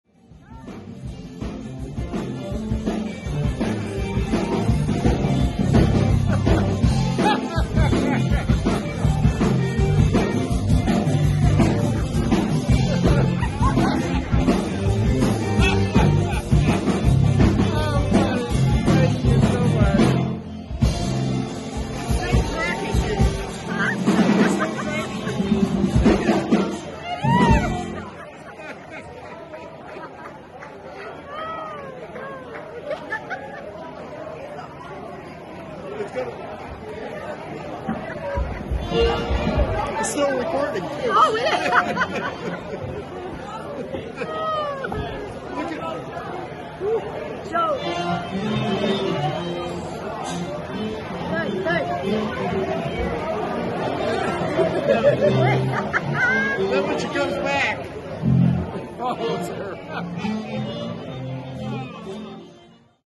A boy's electronic toy in action. Jeffery Broussard at Feed N Seed on 2/18/23
Jeffery Broussard at Feed N Seed on 2/18/23.   For high definition, select the "gear" symbol and then select “hd”, “720” or “1080” if available.